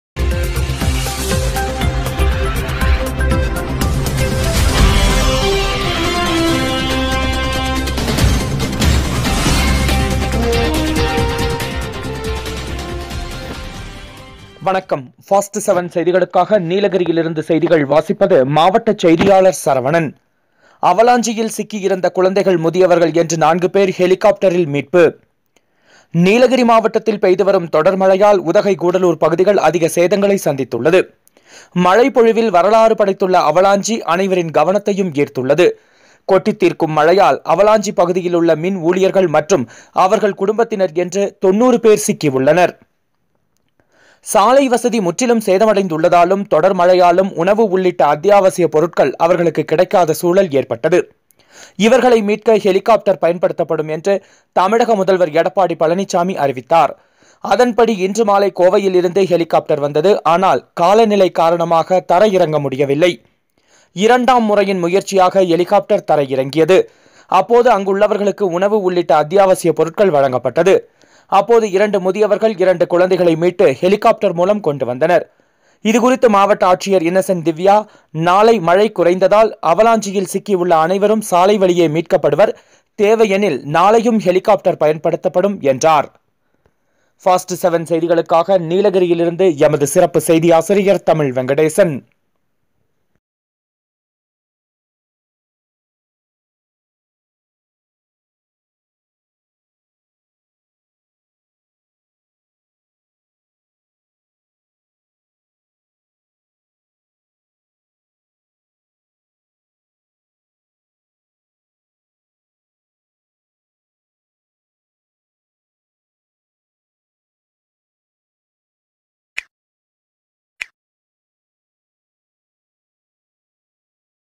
அவலாஞ்சியில் சிக்கி இருந்த குழந்தைகள் முதியவர்கள் என நான்கு பேர் ஹெலிகாப்டரில் மீட்பு நீலகிரி
அவலாஞ்சியில் சிக்கி இருந்த    குழந்தைகள் முதியவர்கள் என நான்கு பேர்  ஹெலிகாப்டரில் மீட்பு

நீலகிரி மாவட்டத்தில் பெய்து வரும் தொடர் மழையால் உதகை கூடலூர் பகுதிகள் அதிக சேதங்கங்களை சந்தித்துள்ளது.

மழை பொழிவில் வரலாறு படைத்துள்ள அவலாஞசி அனைவரின் கவனத்தையும் ஈர்த்துள்ளது.
கொட்டி தீர்க்கும் மழையால் அவலாஞ்சி பகுதில் உள்ள மின் ஊழியர்கள் மற்றும் அவர்கள் குடும்பத்தினர் என 90 பேர் சிக்கி உள்ளனர்.

சாலை வசதி முற்றிலும் சேதமடைந்துள்ளதாலும் தொடர்மழையாலும் உணவு உள்ளிட்ட அத்தியாவசிய பொருட்கள் அவர்களுக்கு கிடைக்காத சூழல் ஏற்ப்பட்டது...

இவர்களை மீட்க ஹெலிகாப்டர் பயன்படுத்தபடும் என தமிழக முதல்வர் எடப்பாடி பழனிசாமி அறிவித்தார் அதன்படி இன்று மாலை கோவையில் இருந்து ஹெலிகாப்டர் வந்தது ஆனால் காலநிலை காரணமாக தரை இறங்க முடியவில்லை .

இரண்டாம் முறையின் முயற்சியாக ஹெலிகாப்டர் தரை இறங்கியது அப்போது அங்குள்ளவர்களுக்கு உணவு உள்ளிட்ட அத்தியாவசிய பொருட்கள் வழங்கப்பட்டது.

அப்போது 2 முதியவர்கள் இரண்டு குழந்தைகளை மீட்டு ஹெலிகாப்டர் மூலம் கொண்டு வந்தனர்.

இது குறித்து கூறிய மாவட்ட ஆட்சியர் இன்னசெண்ட் திவ்யா நாளை மழை குறைந்தால் அவலாஞ்சியில் சிக்கி உள்ள அனைவரும் சாலை வழியே மீட்கபடுவர் தேவை எனில் நாளையும் ஹெலிகாப்டர் பயனபடுத்தபடும் என்றார்.